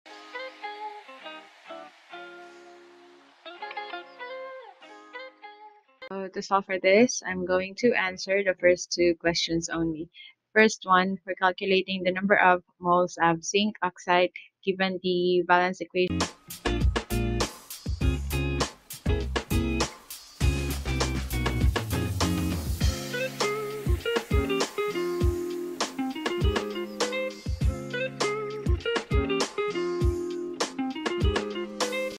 1. Consider the following balanced equation: 2ZnS + 3O2 ? 2ZnO + 2SO2 Determine the number of moles…
1. Consider the following balanced equation: 2ZnS + 3O2 ? 2ZnO + 2SO2 Determine the number of moles of zinc oxide produced when 2.5 mol of zinc sulfide is combined with excess O2. (3T) 2. **Consider the following reaction: (5T) CH4 + O2 ? CO2 + H2O How many grams of carbon dioxide will be produced when 15.0 g of CH4 and 30.0 g of O2 are available for reaction? 3. **Sodium chloride is produced when sodium metal combines with chlorine gas as shown in the following balanced equation: (6T) Na + Cl2 ? NaCl In an experiment, 36.9 g of sodium chloride is produced when 15.9 g of Na and 27.4 g of chlorine are combined. Determine the percentage yield of the product.